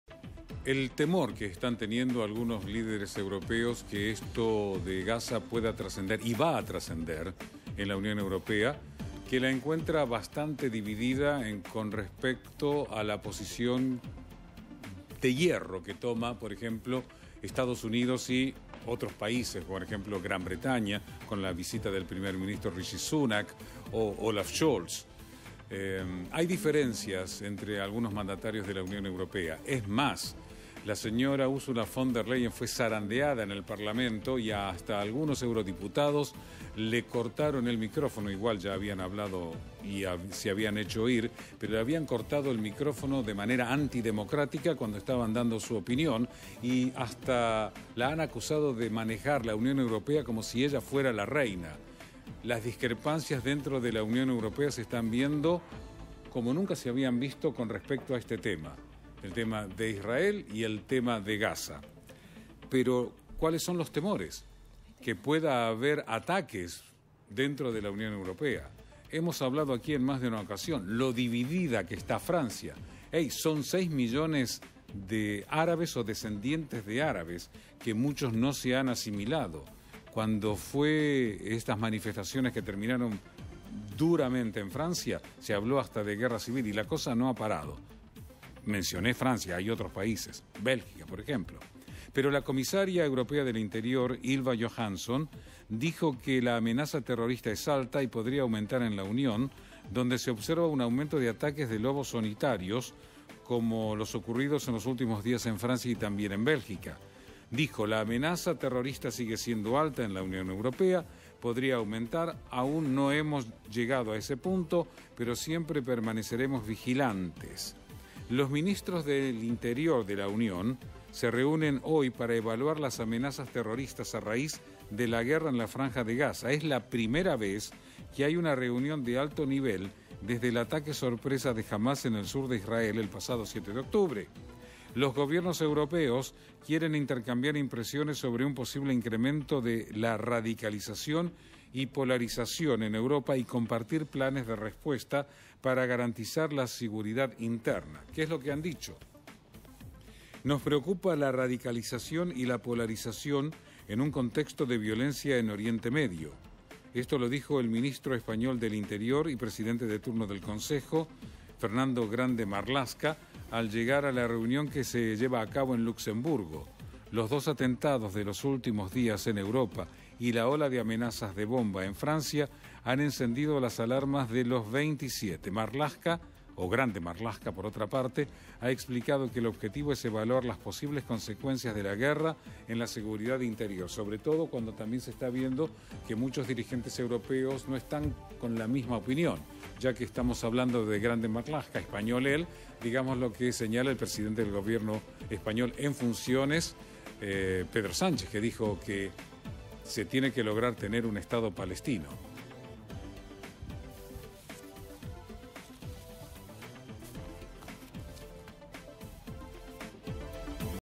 Europa en alerta máxima por terrorismo
👉 Ante el temor de una expansión de ataques terroristas,  varios países europeos comienzan a tomar medidas preventivas tras el conflicto de Medio Oriente.